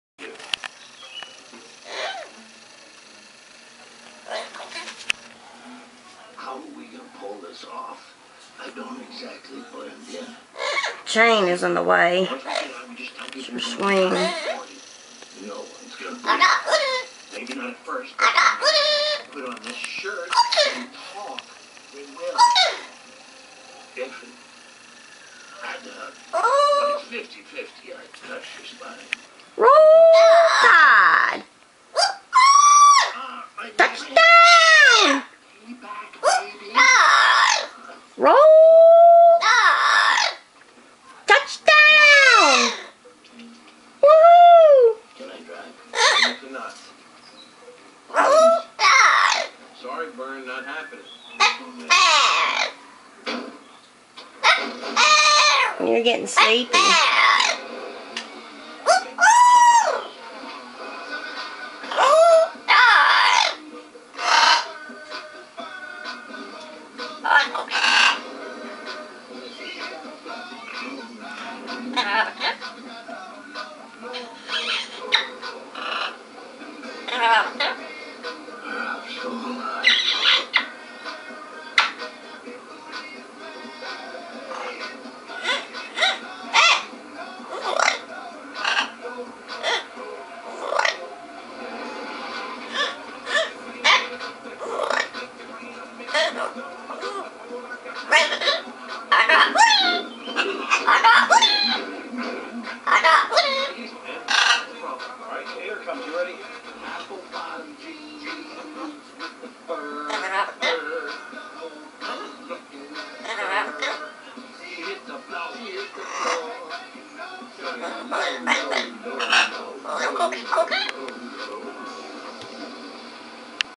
University of Alabama Fan.wmv
One of my Blue Front Amazon's talking while falling asleep.  He loves University of Alabama.